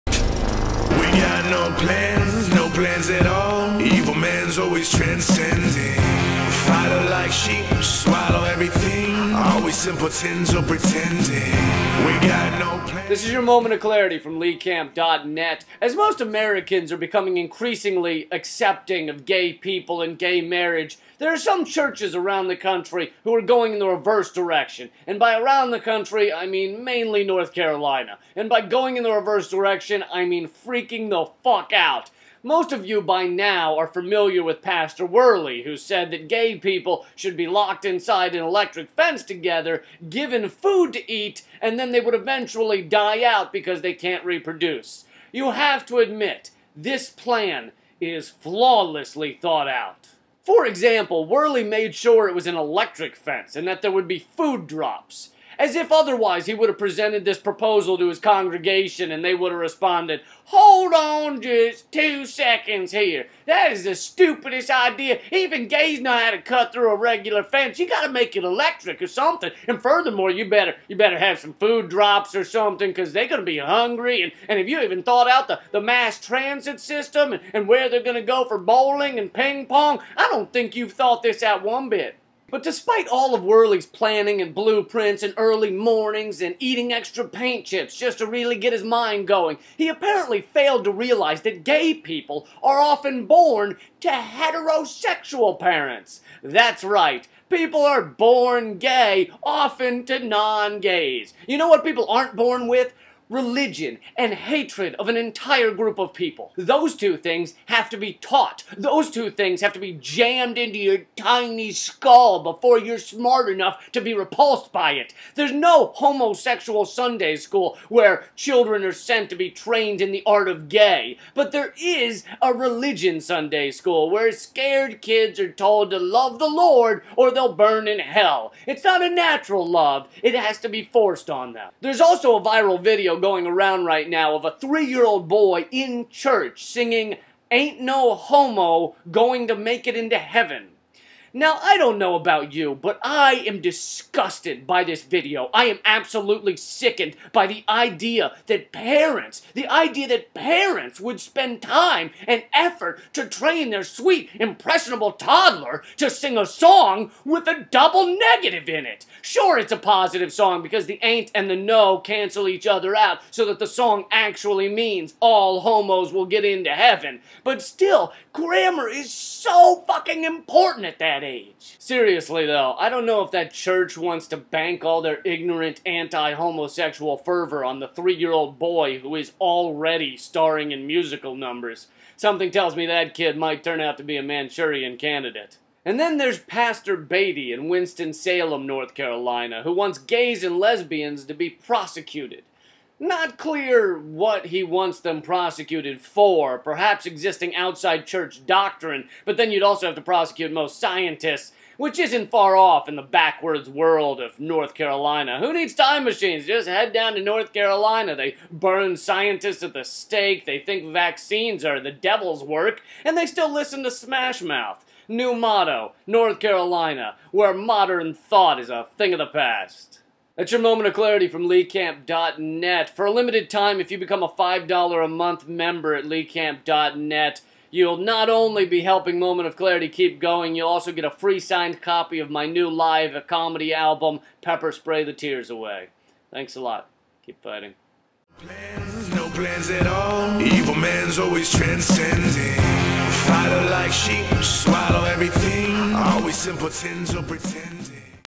"People Are Born Gay, People Are Taught To Hate" - MOC #150
An impressive number of gay hating Church services have come to light recently. ...And most seem to be in North Carolina. What the hell is going on down there?? [more at LeeCamp.net]